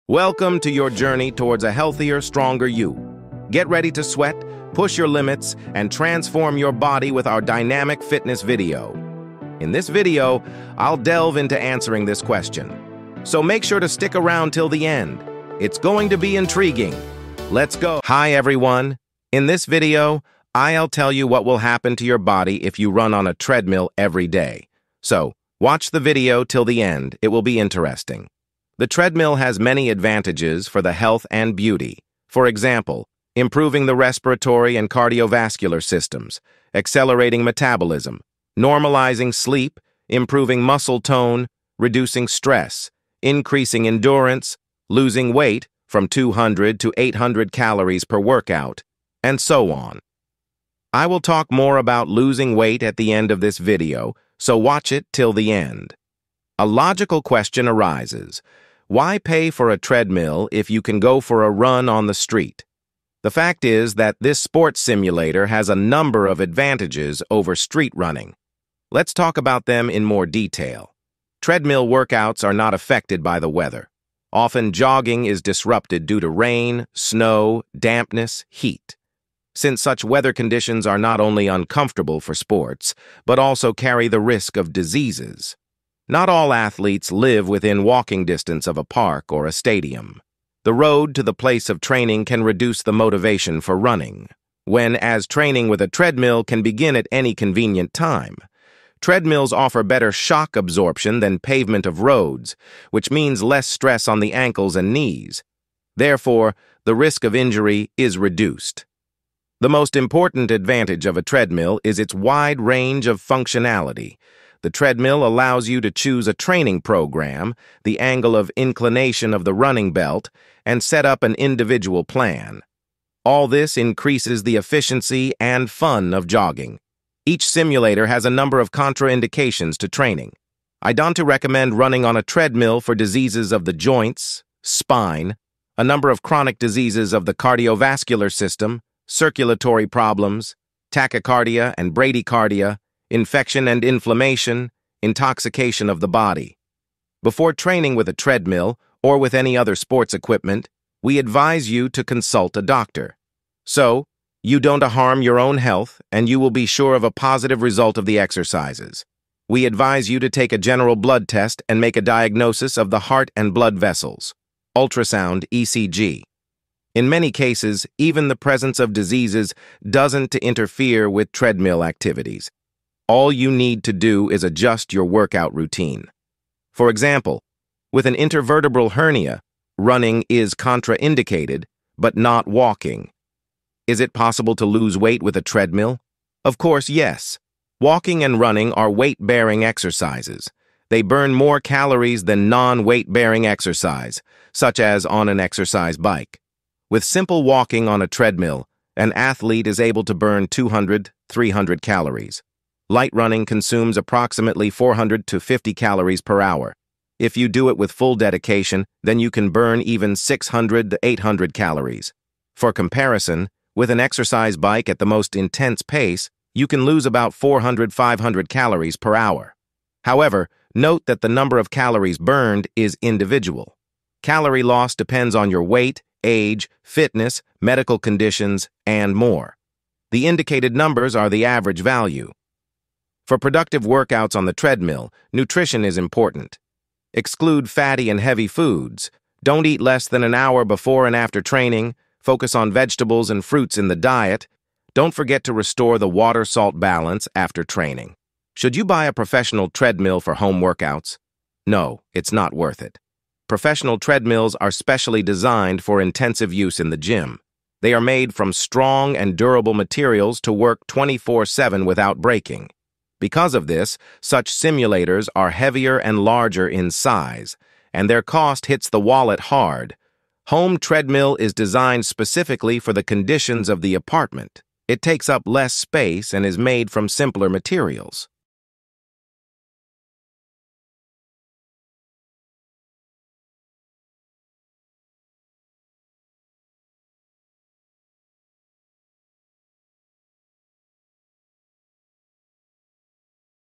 What Happens to Your Body When You Run On a Treadmill Every Day
In this video, I'll tell you what will happen to your body if you run on a treadmill every day, what are the benefits of a treadmill and how to lose weight using treadmill.

Ever wondered what daily treadmill running does to your body? 🏃‍♂️💥 In this video, we explore the incredible benefits and potential drawbacks of making treadmill running a part of your daily routine. Whether you're a seasoned runner or just starting, understanding the impact on your body is crucial for maximizing your fitness gains and avoiding injuries.

In this video, we'll cover:

Cardiovascular Health: Discover how daily treadmill running strengthens your heart and improves circulation.

Weight Management: Learn about the calorie-burning potential and how it aids in weight loss.

Muscle Tone: See which muscles get a workout and how treadmill running can help tone your body.

Mental Health Benefits: Find out how regular running can boost your mood and reduce stress.

Potential Risks: Understand the possible downsides, including joint stress and injury prevention tips.

Why You Should Watch:
Gain insights into how consistent treadmill running can transform your health.
Get expert tips on making the most out of your treadmill workouts.
Learn about safe practices to prevent overuse injuries.